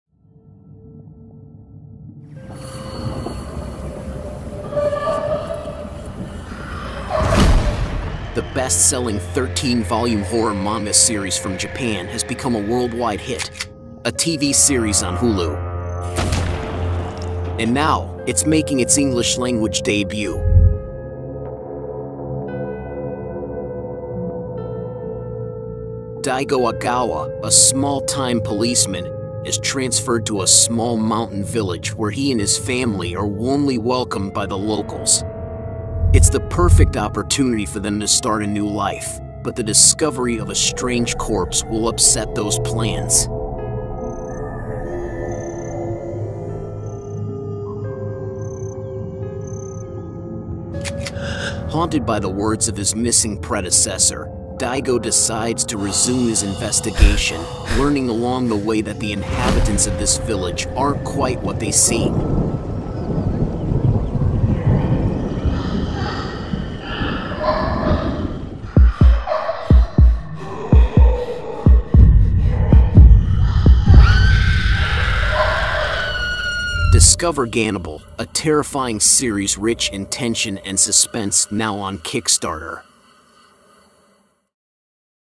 GANNIBAL Kickstarter Campaign trailer.
ABLAZE Brings Masaaki Ninomiya’s Horror Manga Series to Kickstarter in Time for Halloween!

A terrifying thriller series rich in tension and suspense, GANNIBAL has been adapted into a live-action TV series, directed by Shinzo Katayama (director of J-horror movie Missing) that currently streams on Disney+/Hulu and stars Yuya Yagira, Sho Kasamatsu, and Riho Yoshioka. In the series, Daigo Agawa is a resident police officer who has been recently assigned to the remote mountain village of Kuge. He is initially warmly welcomed by the villagers, but with the suspicious death of an old woman, he gradually becomes aware of the anomaly of the village and begins to suspect that “The people in this village are eating people.”

ABOUT THE MANGAKA: MASAAKI NINOMIYA is a manga artist based in Tokyo, Japan. Despite his brief career, he has received accolades that have helped to catapult him to worldwide recognition. He received an honorable mention at the 68th Chiba Tetsuya Award for his work Revoltech Taniguchi, as well as the Second Grand Prize of THE GATE contest for the Domestic Bliss comic. His first serialized manga story was "Chousou no Babel" in 2016 and the serialization of "Gannibal" began in 2018.